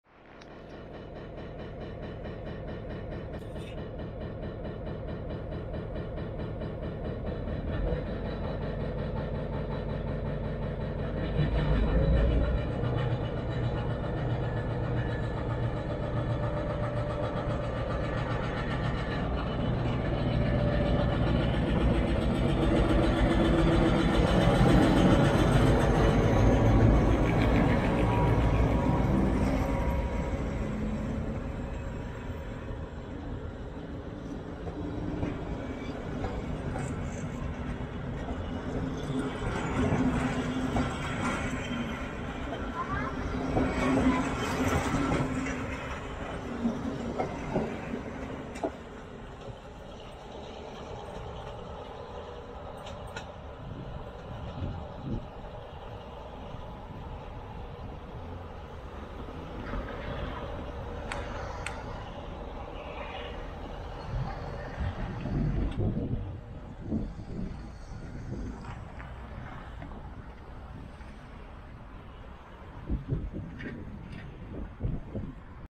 M61 006 Nohab + BDdf (BDt) 149 indul Szurdokpüspökiből, 2022. 05. 29. [4K]
M61 006 Nohab + BDdf (BDt) 149 indul az 5833 számú Somoskőújfalu - Hatvan személyvonattal Szurdokpüspökiből, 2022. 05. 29-én.
M61 006 Nohab + BDdf 149 departs from Szurdokpüspöki (Hungary), with number 5833 Somoskőújfalu (Hungary) - Hatvan (Hungary) passenger-train, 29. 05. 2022.
Video.: Petrás Bence